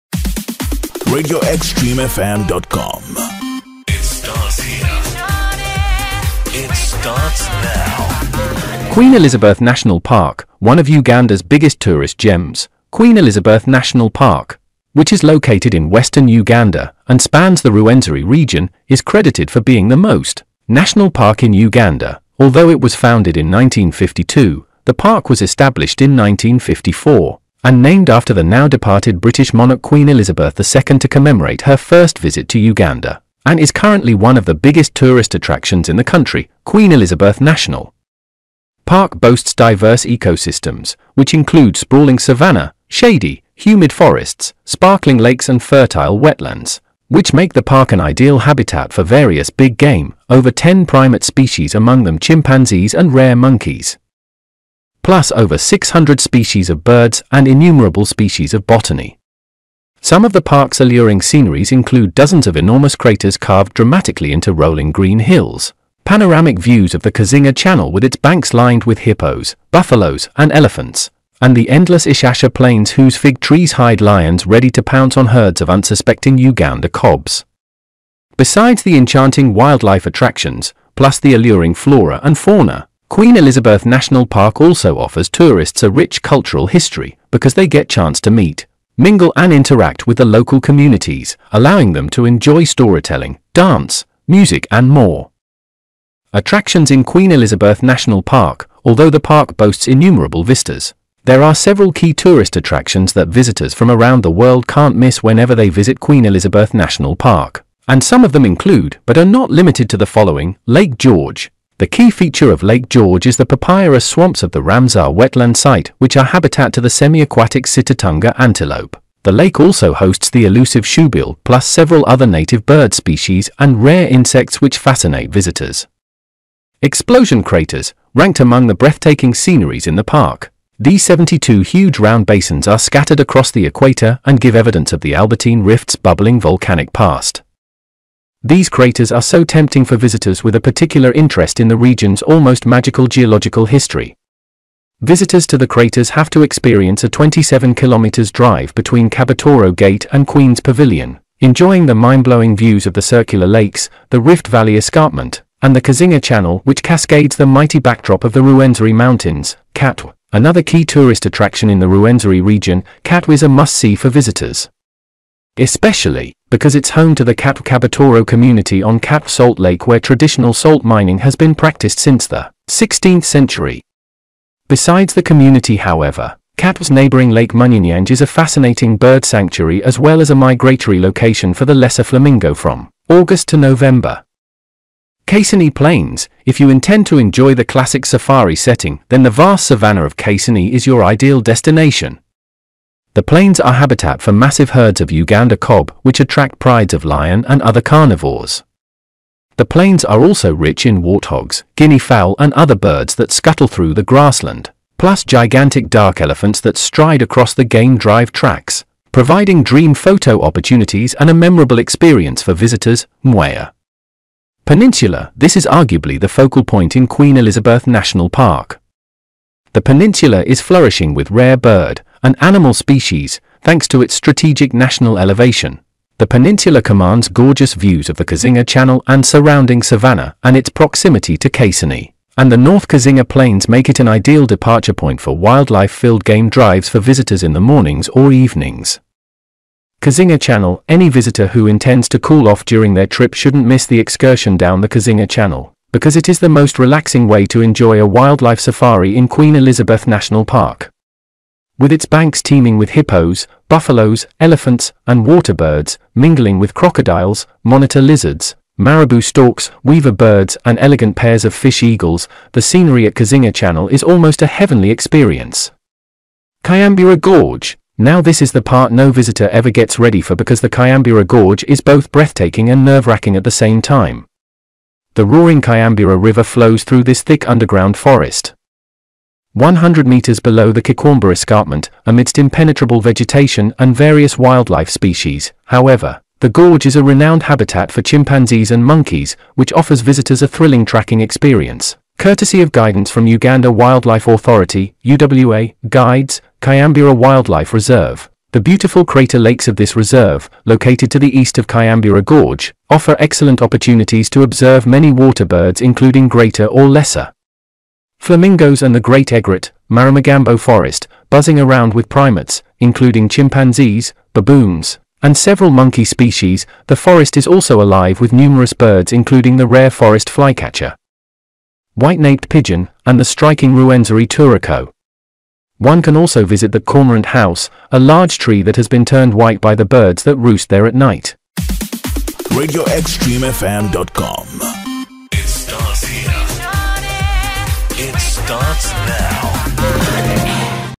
Visit Queen Elizabeth National Park One of Uganda's biggest tourist gems
Queen Elizabeth National Park

One of Uganda's biggest tourist gems, Queen Elizabeth National Park, which is located in western Uganda, and spans the Rwenzori Region, is credited for being the most- National Park in Uganda.

Although it was founded in 1952, the park was established in 1954, and named after the now departed British Monarch Queen  Elizabeth II to commemorate her first visit to Uganda and is currently one of the biggest tourist attractions in the country.

Queen Elizabeth National Park boasts diverse ecosystems, which include sprawling savanna, shady, humid forests, sparkling lakes and fertile wetlands, which make the park an ideal habitat for various big game, over 10 primate species among them chimpanzees and rare monkeys, plus over 600 species of birds and innumerable species of botany.

Some of the park's alluring sceneries include dozens of enormous craters carved dramatically into rolling green hills, panoramic views of the Kazinga channel with its banks lined with hippos, buffalos and elephants, and the endless Ishasha Plains whose fig trees hide lions ready to pounce on herds of unsuspecting Uganda kobs.

Besides the enchanting wildlife attractions, plus the alluring flora and fauna, Queen Elizabeth National park also offers tourists a rich cultural history, because they get chance to meet, mingle and  interact with the local communities, allowing them to enjoy storytelling, dance, music and more.

Attractions In Queen Elizabeth National Park

Although the park boasts innumerable vistas, there are several key tourist attractions that visitors from around the world can't miss whenever they visit Queen Elizabeth National Park and some of them include but are not limited to the following;

Lake George

The key feature of Lake George is the papyrus swamps of the Ramsar wetland site which are habitat to the semi-aquatic Sitatunga antelope.  The lake also hosts the elusive shoebill plus several other native bird species and rare insects which fascinate visitors.

Explosion Craters
Ranked among the breathtaking sceneries in the park, these 72 huge round basins are scattered across the equator and give evidence of the Albertine Rift’s bubbling volcanic past.

These craters are so tempting for visitors with a particular interest in the region's almost magical geological history.

 Visitors to the craters have to experience a 27km drive between Kabatoro gate and Queens Pavilion, enjoying the mind-blowing views of the circular lakes, the Rift Valley escarpment, and the Kazinga channel which cascades the mighty backdrop of the Rwenzori Mountains.

Katwe

Another key tourist attraction in the Rwenzori Region, Katwe is a must-see for visitors, especially because it's home to the Katwe-Kabatoro community on Katwe Salt Lake where traditional salt mining has been practiced since the 16th century. Besides the community however, Katwe's neighboring Lake Munyanyange is a fascinating bird sanctuary as well as a migratory location for the lesser flamingo from August to November.

Kasenyi Plains

If you intend to enjoy the classic Safari setting then the vast savannah of Kasenyi is your ideal destination!
The plains are habitat for massive herds of Uganda kob which attract prides of lion and other carnivores.

The plains are also rich in warthogs, guinea fowl and other birds that scuttle through the grassland, plus gigantic dark elephants that stride across the game drive tracks, providing dream photo opportunities and a memorable experience for visitors.

Mweya Peninsula

This is arguably the focal point in Queen Elizabeth National Park. The peninsula is flourishing with rare bird
and animal species.

Thanks to its strategic national elevation, the peninsula commands gorgeous views of the Kazinga Channel and surrounding savanna and its proximity to Kasenyi and the North Kazinga plains make it an ideal departure point for wildlife-filled game drives for visitors in the mornings or evenings.

Kazinga Channel

Any visitor who intends to cool off during their trip shouldn't miss the excursion down the Kazinga channel, because it is the most relaxing way to enjoy a wildlife safari in Queen Elizabeth National Park.

With its banks teeming with hippos, buffalos, elephants, and water birds, mingling with crocodiles, monitor lizards, Marabou storks, weaver birds and elegant pairs of fish eagles, the scenery at Kazinga Channel is almost a heavenly experience!